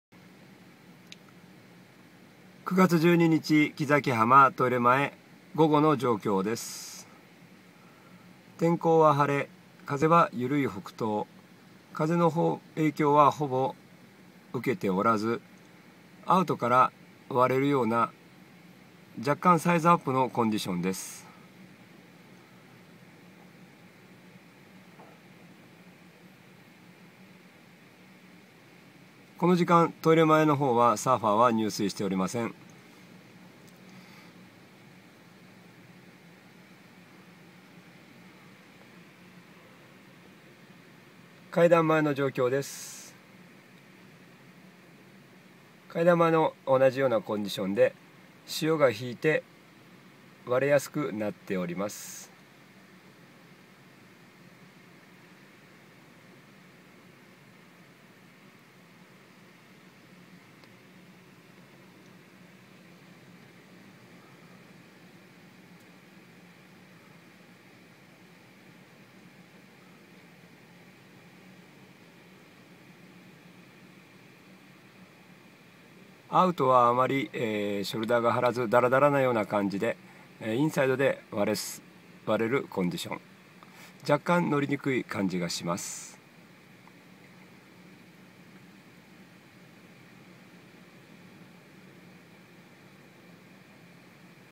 木崎浜 9/12 15:00
天候は晴れ、風はゆるい北東。 風の影響は余りなくてアウトからも割れる波が入ってきています！！
腰・セット胸くらいです・・・
 大潮 干潮は12:12、満潮は18:29